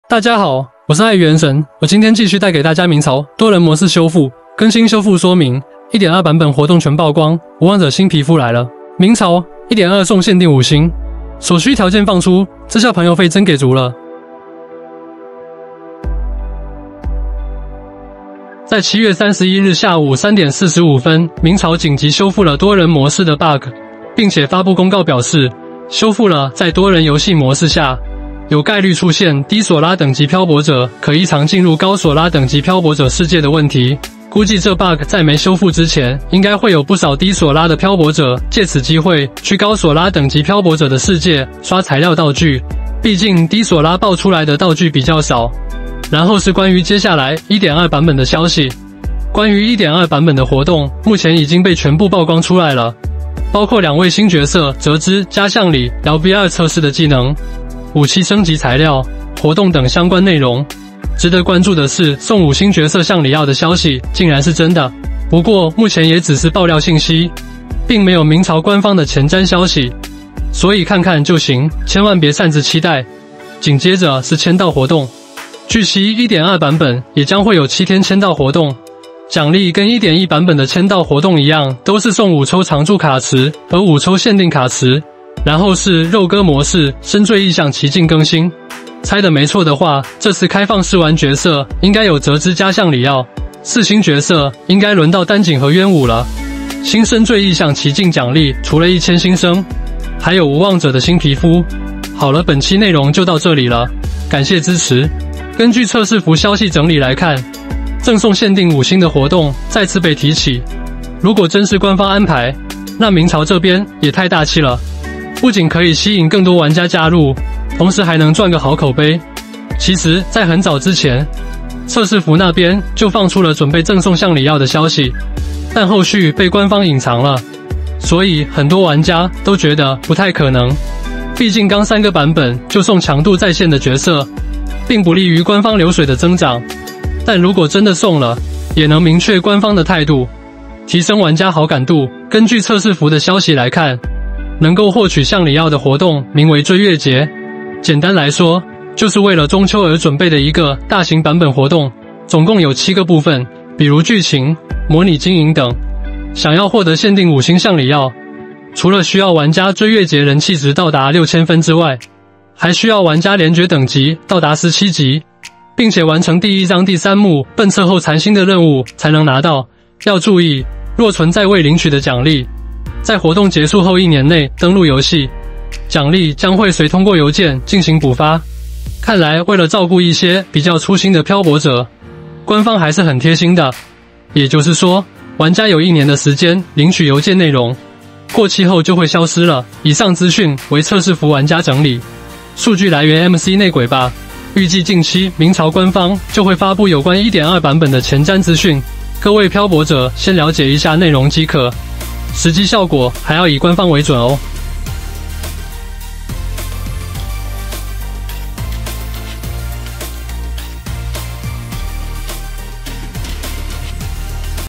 鳴潮：多人模式修復，更新修復說明，1.2版本活動全曝光，無妄者新皮膚來了，鳴潮：1.2送限定五星！所需條件放出，這下朋友費真給足了
Mingchao: Multiplayer mode repair, update repair instructions, 1.2 version activities are fully exposed, Wuwangzhe new skin is coming, Mingchao: 1.2 gives limited five stars! The required conditions are released, this time the friend fee is really enough